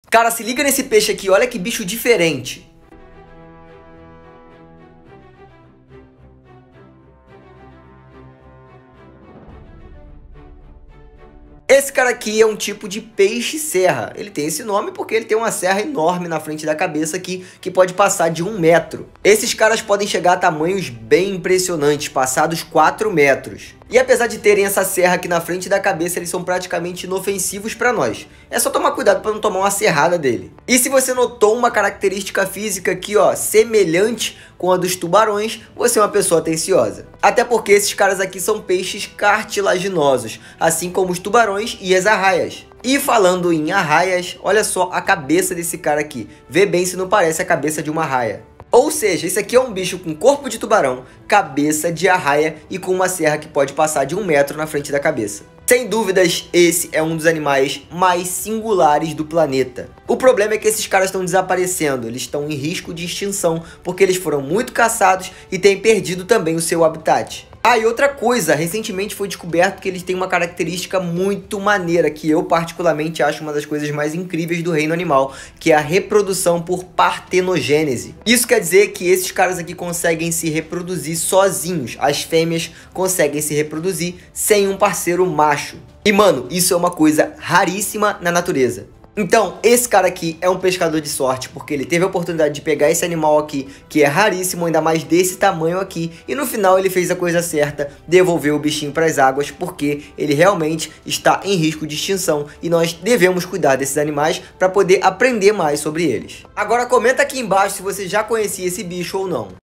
BICHO BIZARRO TEM SERRA NA CABEÇA E É MISTURA DE ARRAIA E TUBARÃO! PEIXE-SERRA | Biomesquita
Gostou do vídeo? No instagram tem MUITO mais detalhes de todo meu trabalho, cola lá